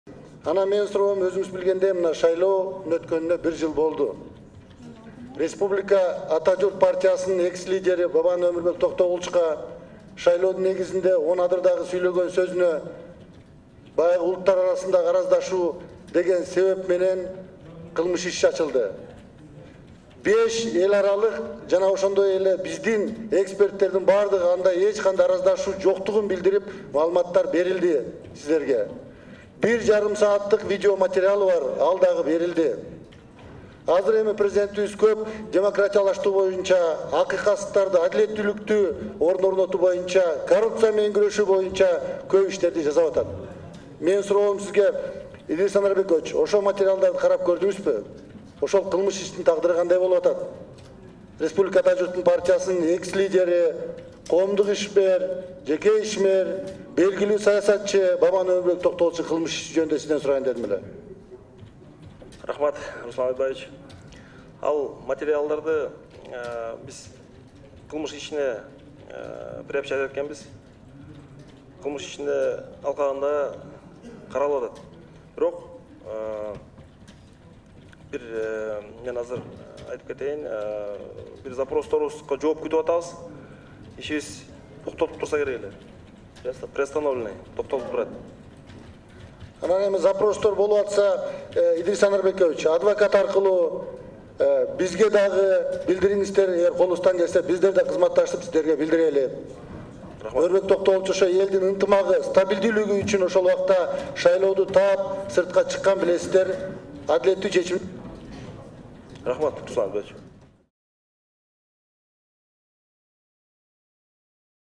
Бабановдун кылмыш иши тергелбей токтоп турат
Өмүрбек Бабановго карата козголгон кылмыш иш учурда токтоп турат. Бул тууралуу Жогорку Кеңештин жыйынында УКМКнын төрагасы Идрис Кадыркулов билдирди.